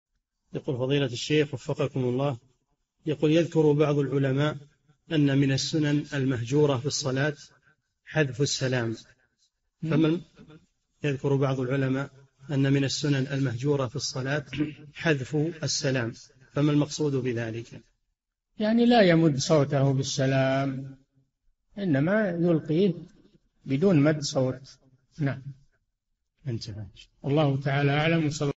[3814- 4717] ما المقصود بقول العلماء إن من السنن المهجورة في الصلاة حذف السلام؟ - الشيخ صالح الفوزان
"[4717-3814] يذكر بعض العلماء أنّ من السنن المهجورة في الصلاة حذف السلام فما المقصود بذلك؟
الجَوَابُ: يعني لا يمد صوته بالسلام وإنما يلقيه بدون مدّ صوت. واللهُ تعالى أعلم، وصلى الله وسلم على نبينا محمد

للاشتراك في قناة الشيخ صالح الفوزان
فتاوى المجالس في تفسير المفصل

للاستفسار والإقتراحات والدعم
رئيس الفريق
لمشروع كبار العلماء